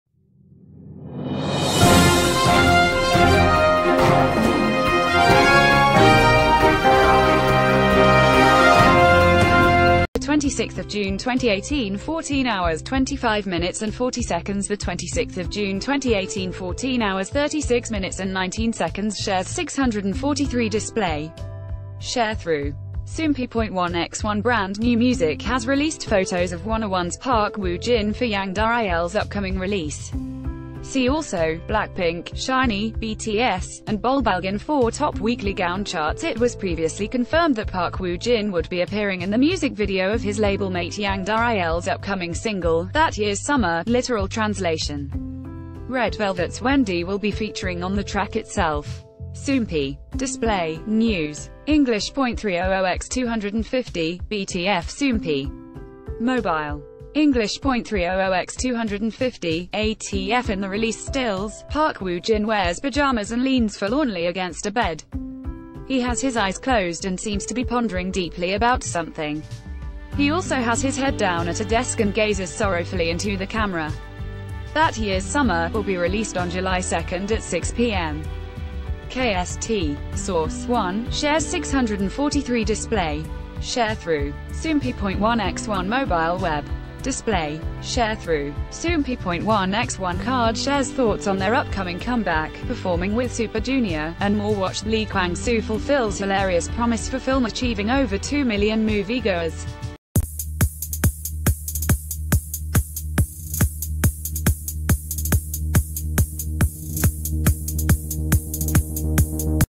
Wanna One's Park Woo Jin Stars In MV Photos For Yang Da Il's New Release With Red Velvet's Wendy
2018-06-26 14:25:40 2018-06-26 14:36:19 Shares 643 Display. Sharethrough. Soompi.1x1 Brand New Music...